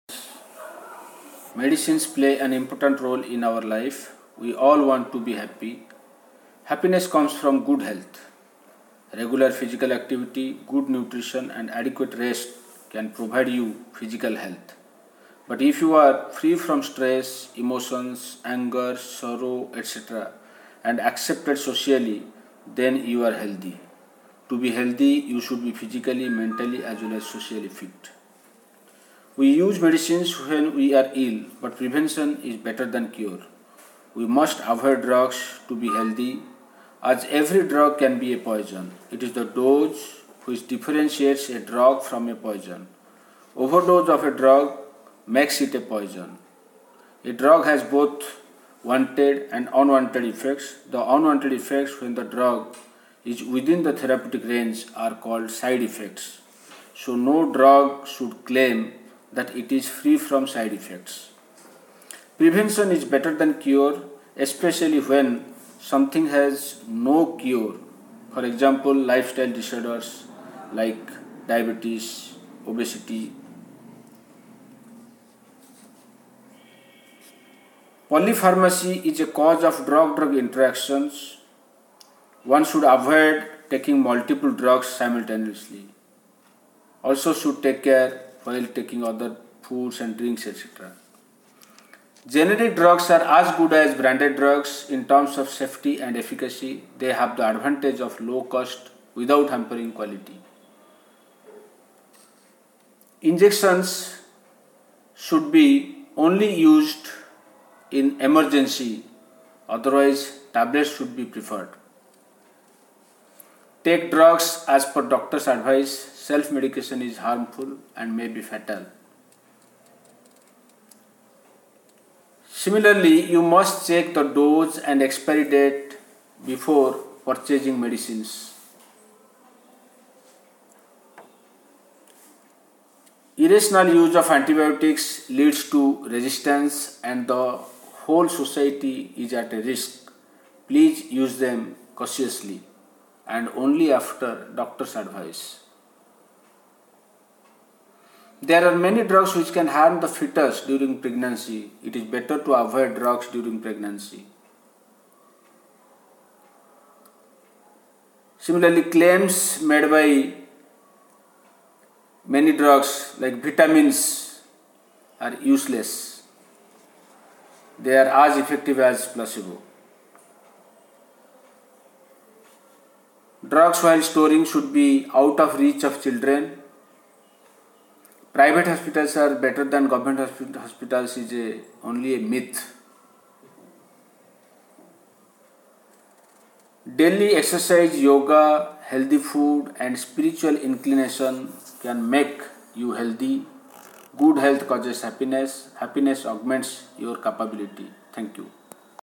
Know your medicines
A person is said to be healthy when he/she is physically, mentally as well as socially fit. Regular physical activity, good nutrition & adequate rest can provide you physical health. But if you are free from stress, emotions, anger, sorrow etc. & accepted socially then only you are healthy. Prevention is better than cure. So, one must avoid drugs to be healthy. All drugs have both wanted effects and unwanted effects. The use of medicines is not always beneficial but also associated with risk. There are certain myths about medicines which one must know to take care of their health. As a Pharmacist, I would like to clarify few myths by addressing the issues such as branded drugs Vs generic drugs; injections Vs  tablets, self medication, poly pharmacy, drug interactions, side effects, toxicity, expiry date, irrational use of drugs, drugs in pregnancy, storage of drugs, Govt. Vs Private hospitals, life style disorders etc. This will help you for safe use of medicines to lead a healthy life. In addition, daily exercise, yoga, healthy food & spiritual inclination can make you healthy. Good Health causes happiness. Happiness augments your capability.